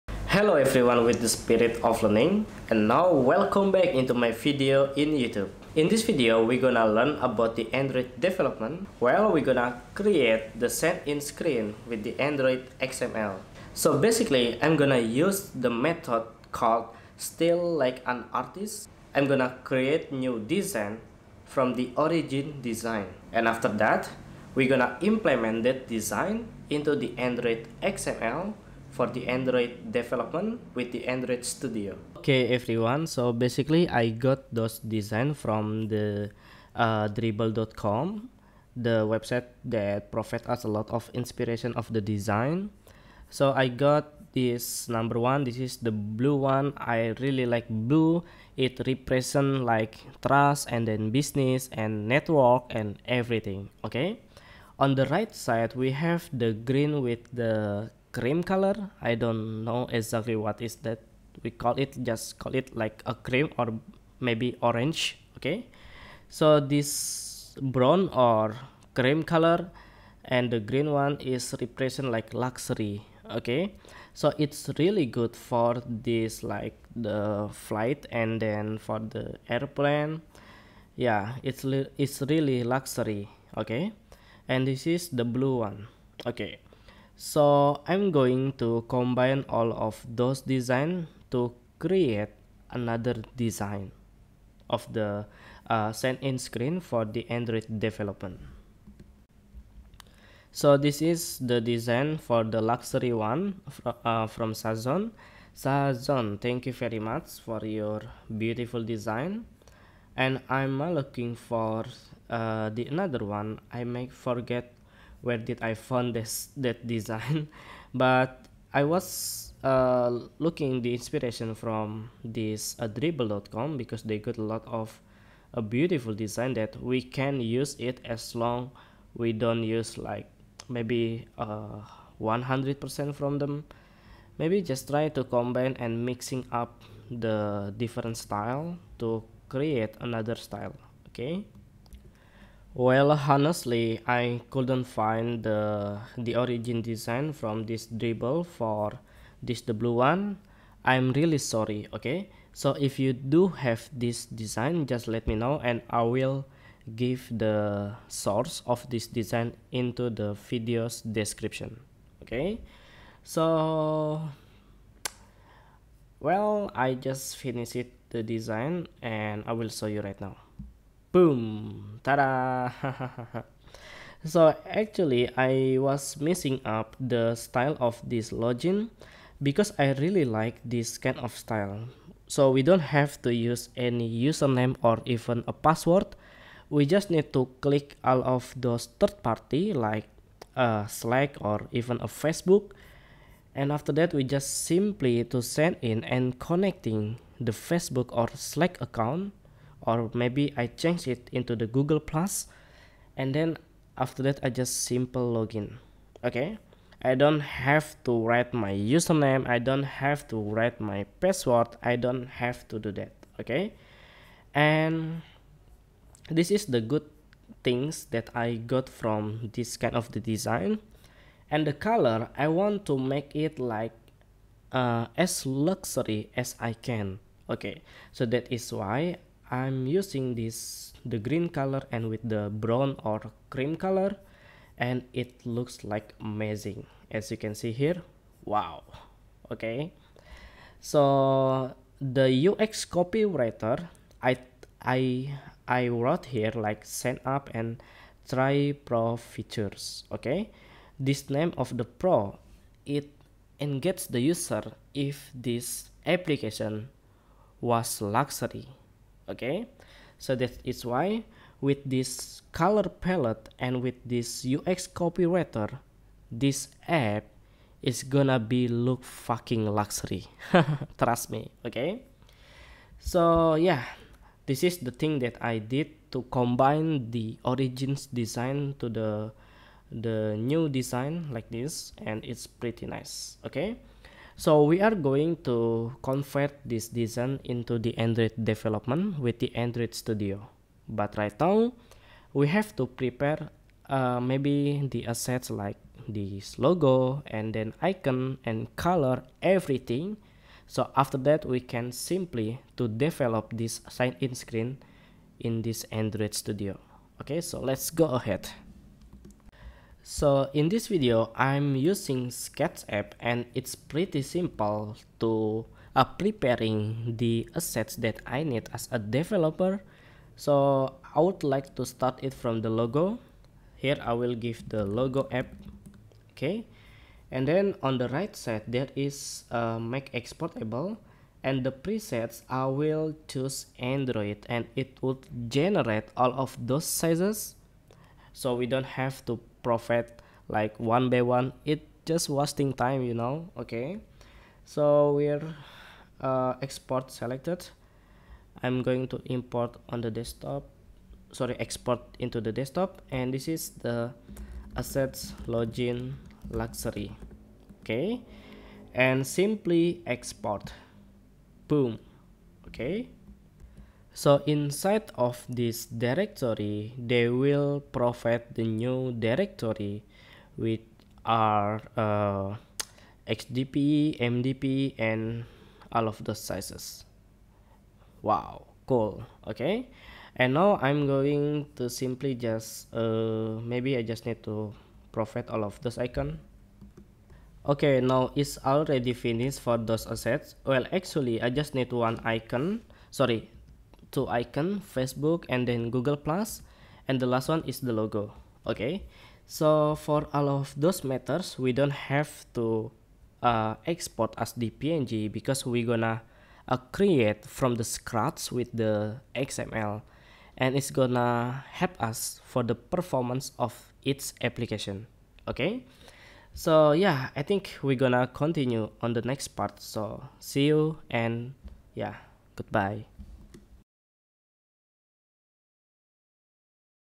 Luxury Sign In Screen — #1 — Android Studio Tutorial
Hi everyone, I am Angga Risky and in this video, we are gonna learn how to design the Sign In screen using Sketch and convert the UI to code with Android Studio for Android development.

🕹My Gear Set:
Sony A6500
Sigma 16mm
MacBook Pro 2030
Samson Mic C01U-Pro